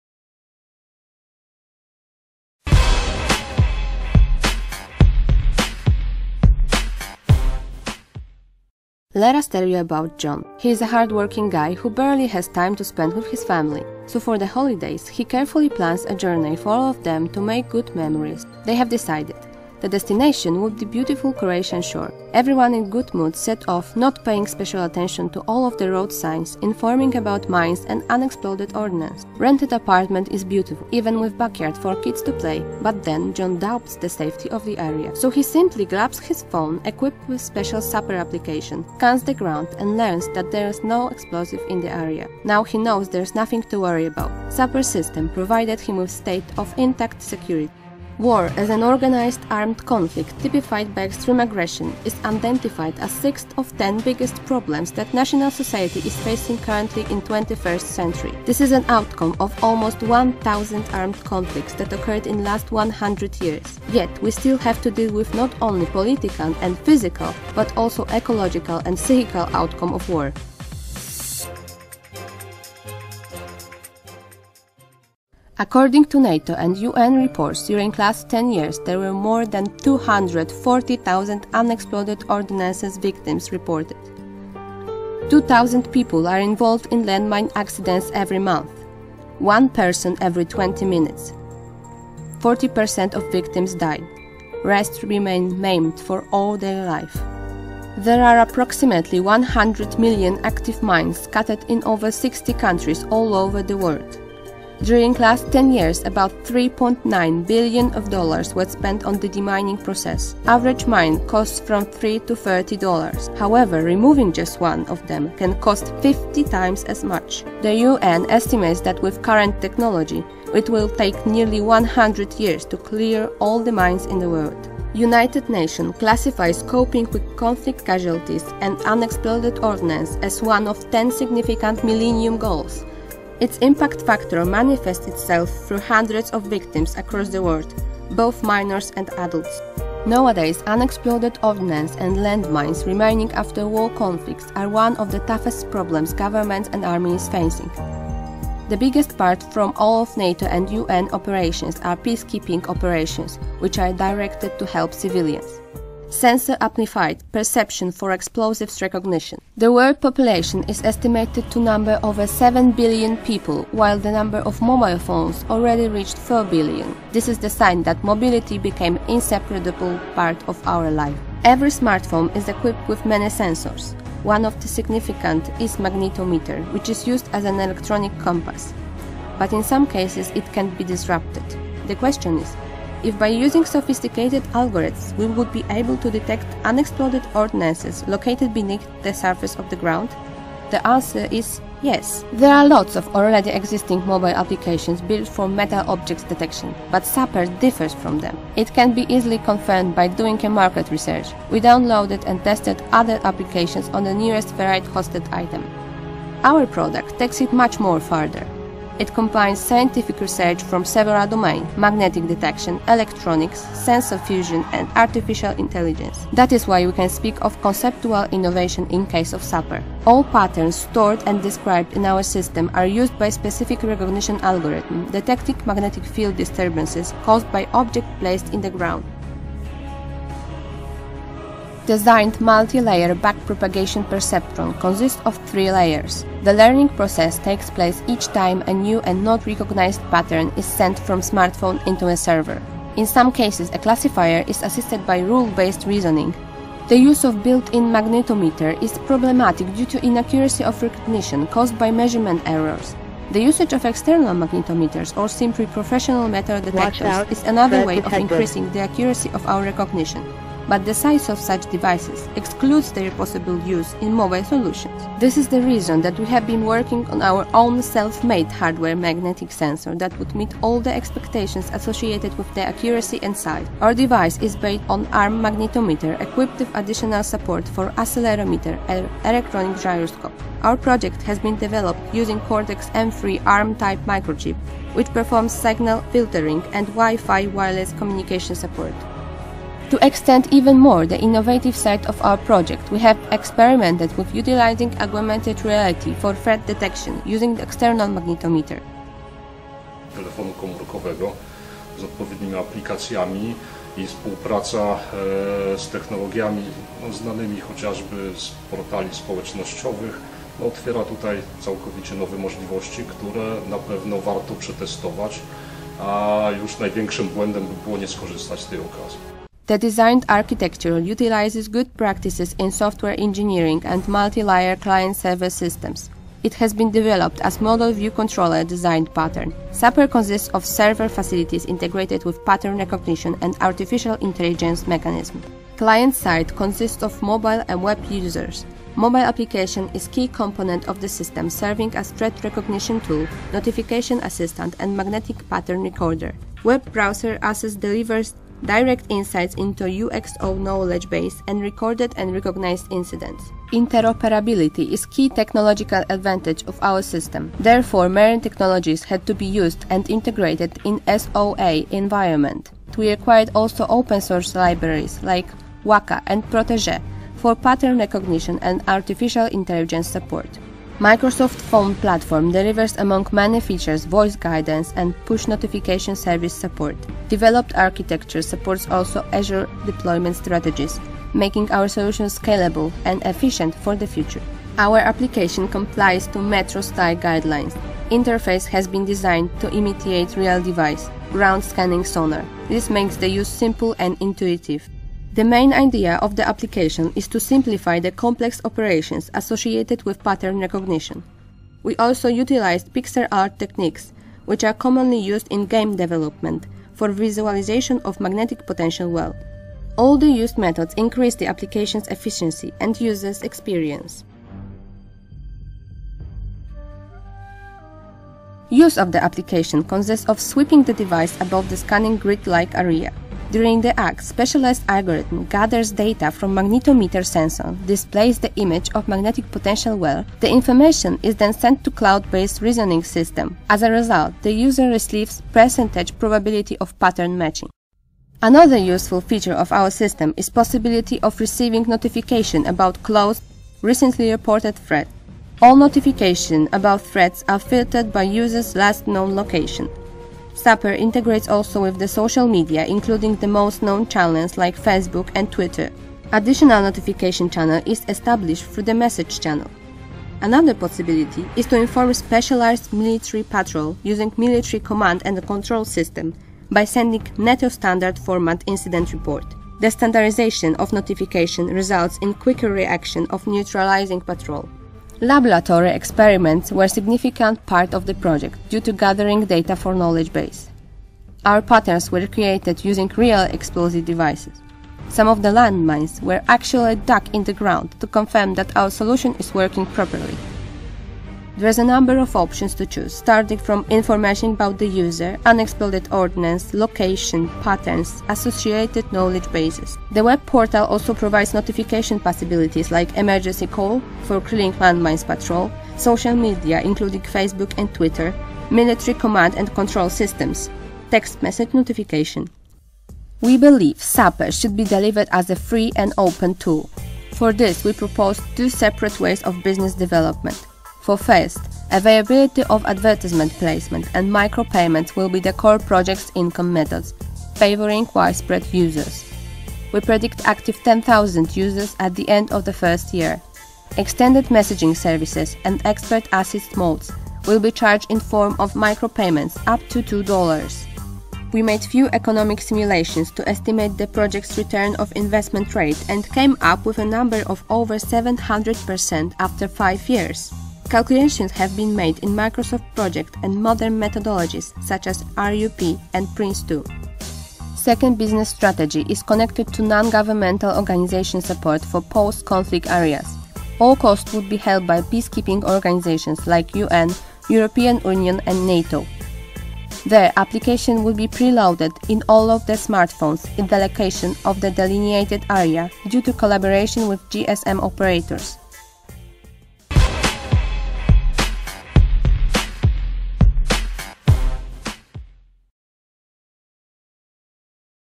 SAPER - Sensor Amplified Perception for Explosives Recognition - Finals
LATEST Screencast - Enjoy

SAPER - Sensor Amplified Perception for Explosives Recognition
SAPER is a mobile solution which identifies hidden improvised explosive devices and landmines utilizing magnetic field distortion. SAPER transforms Smartphone into a personal portable dangerous explosives detector and a ground scanner which can be successfully adopted in real life scenarios by civilians.

from Military University of Technology (Wojskowa Akademia Techniczna)
Cybernetics Faculty (Wydział Cybernetyki)